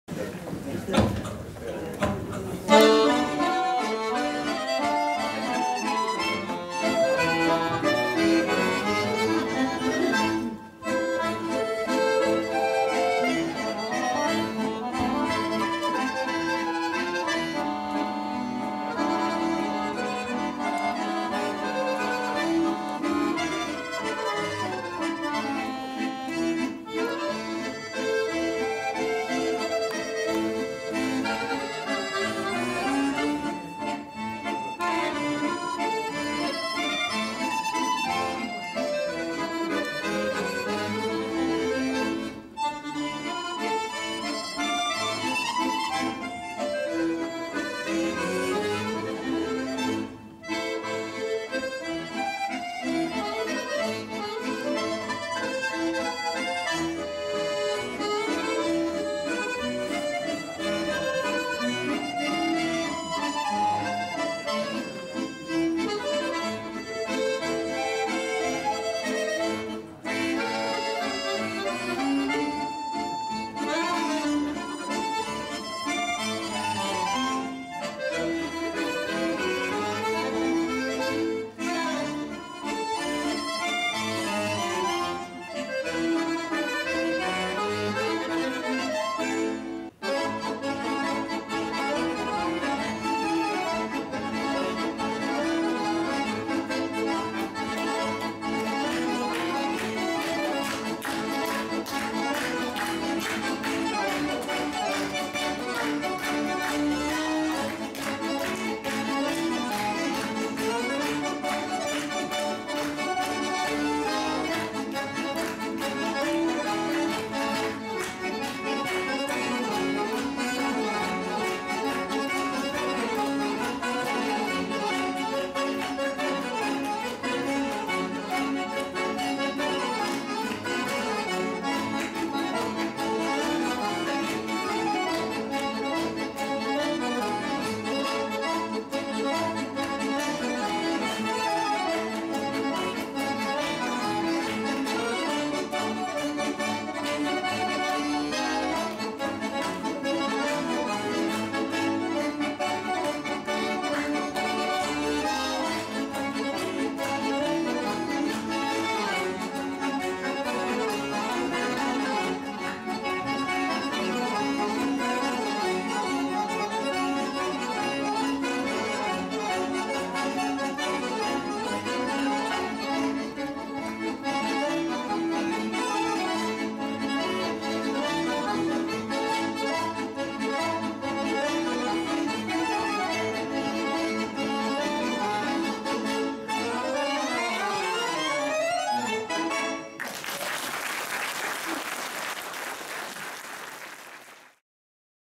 Accordion - Spelmansmusik med Ove och Jari på Grand 29.10.2017
Dragspelskonsert på Grand i Borgå, Finland.
Harmonikkakonsertti Porvoossa.